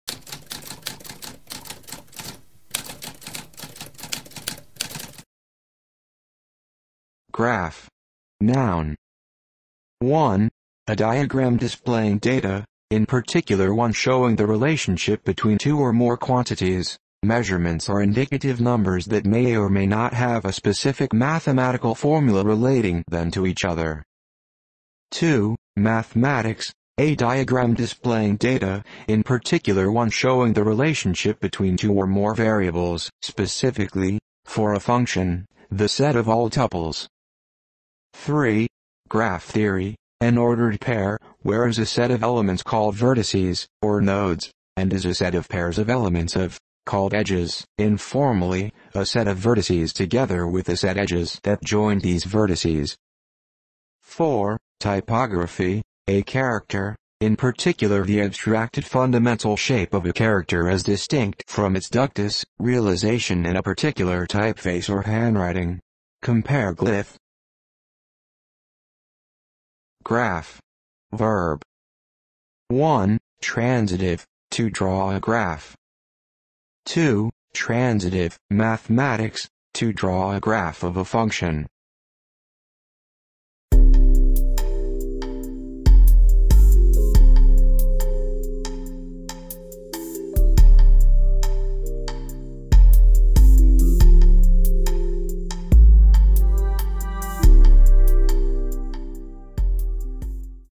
What does graph mean?
A spoken definition of graph.

Intro Sound:
Typewriter - Tamskp